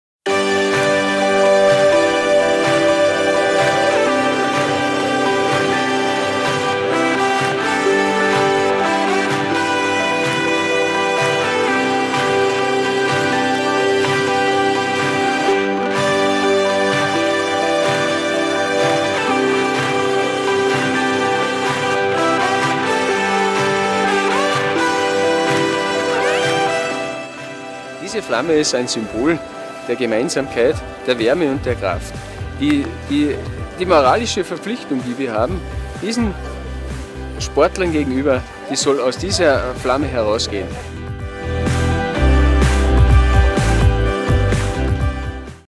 Highlights Olympisches Feuer I Ramsau/Dachstein, 19.03.2017
Am Sonntag, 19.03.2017, wurde das Olympische Feuer auch in der Ramsau/Dachstein angezündet.

In Ramsau/Dachstein the Olympic torch was lit at sunday, march 19th 2017.